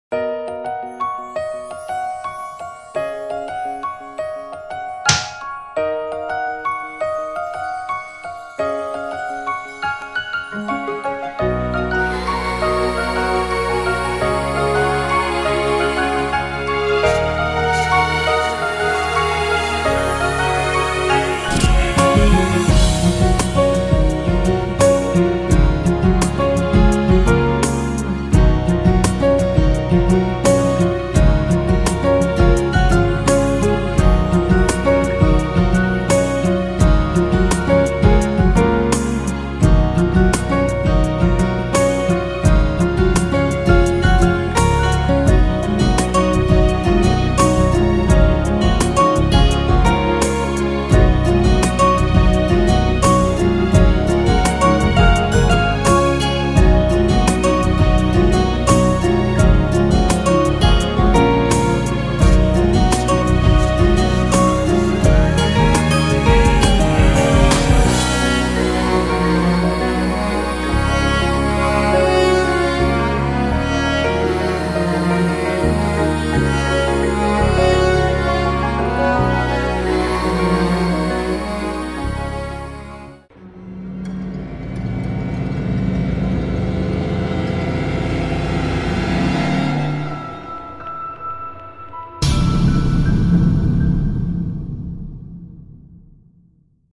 [サイコパス安西] スラムダンク「バスケがしたいです」の名場面に、サイコパス安西登場‼
スラムダンクの三井寿「バスケがしたいです」の名場面風の動画がホラーで蘇る。興味のある方は是非ご覧ください。